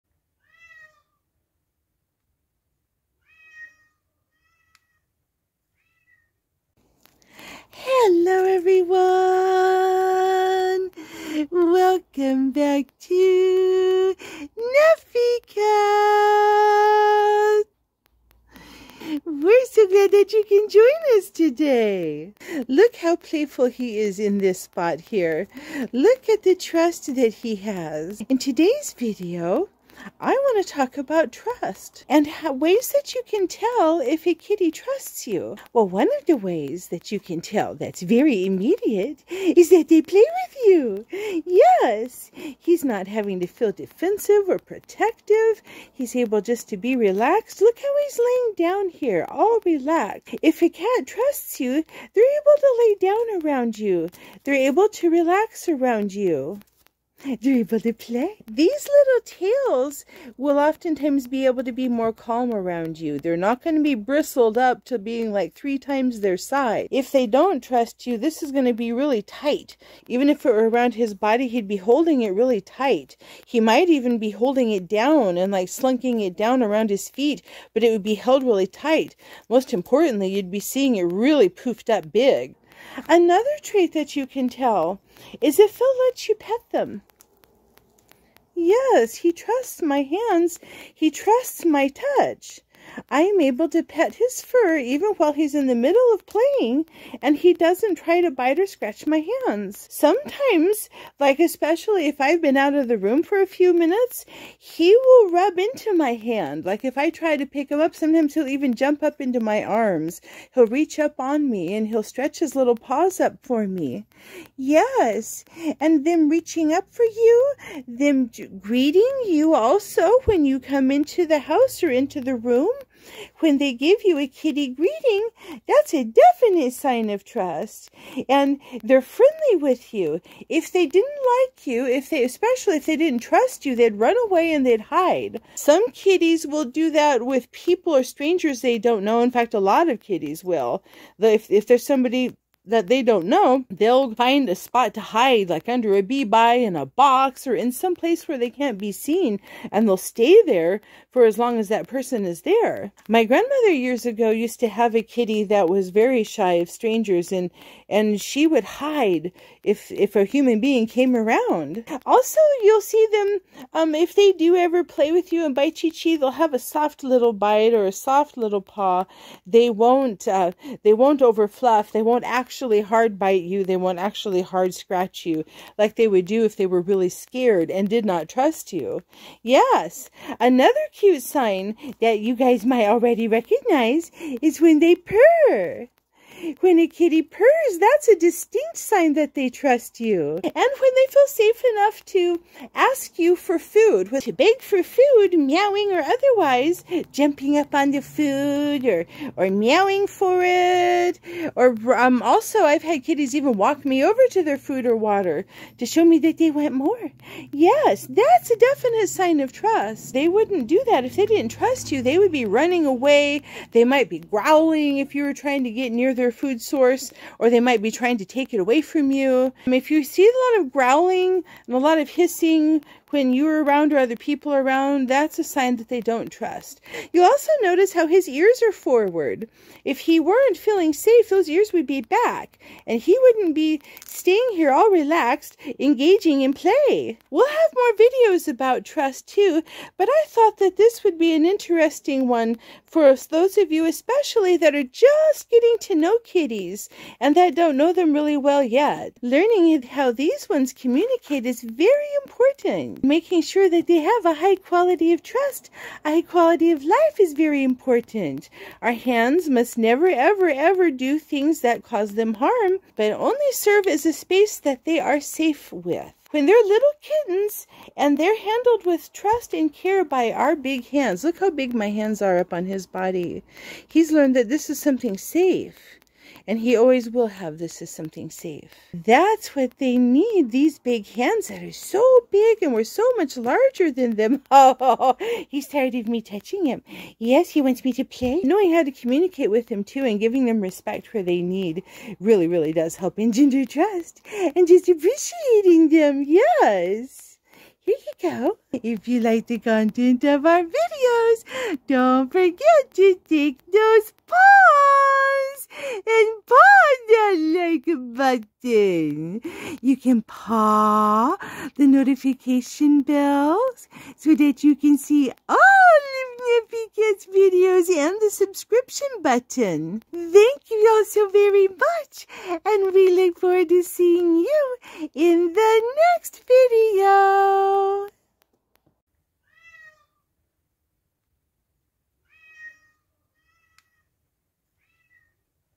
How To Tell If A Cat Trusts You
How can you know that a cat trusts you? We talk about several different signs that signal a cat's trust in us. Learning what those are is valuable for anyone who has a cat.